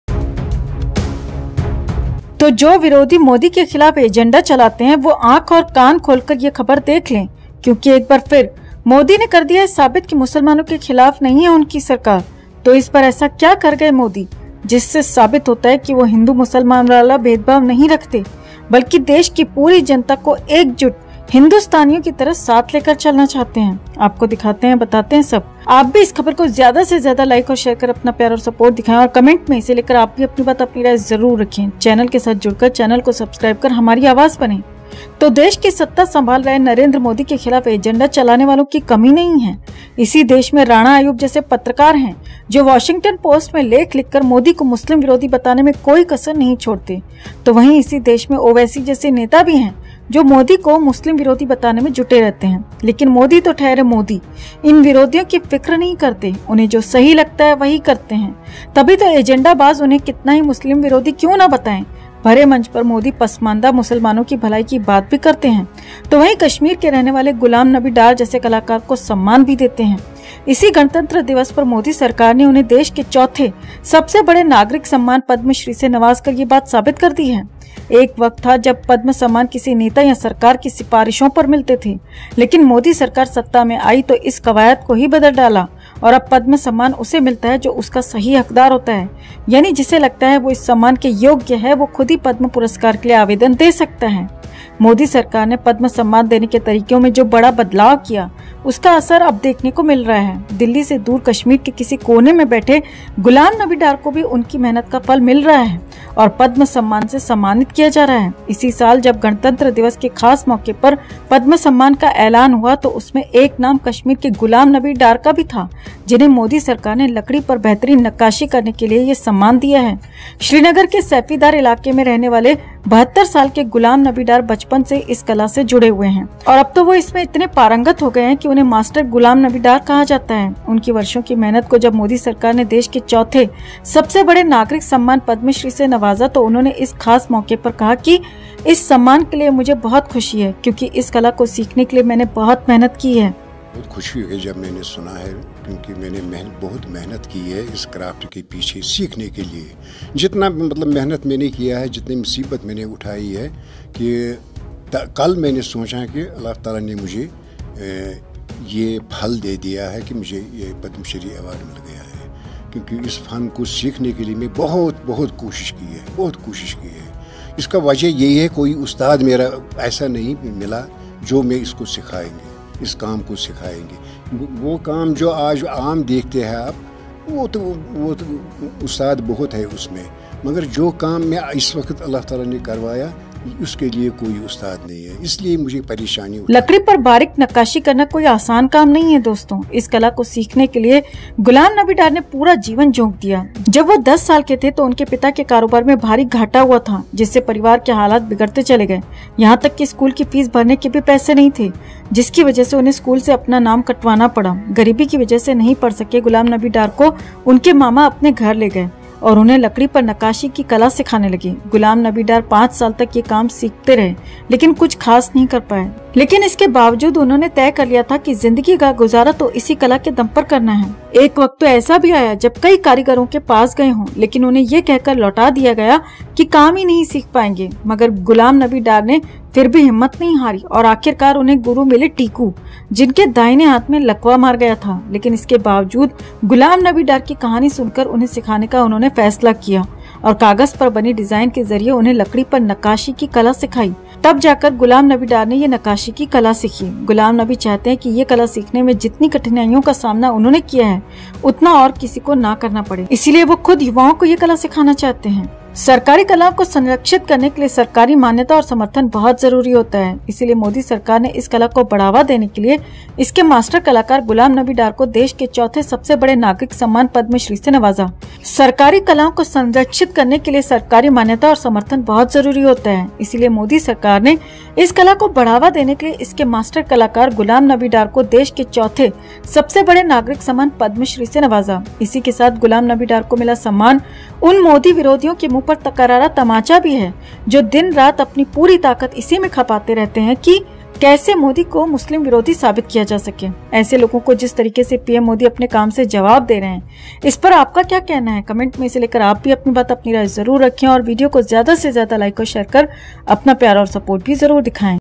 ये खबर देख के आप ही करें फैसला, क्या मुस्लिम विरोधी हैं मोदी?
खबर देख के आप ही करें फैसला, क्या मुस्लिम विरोधी हैं मोदी?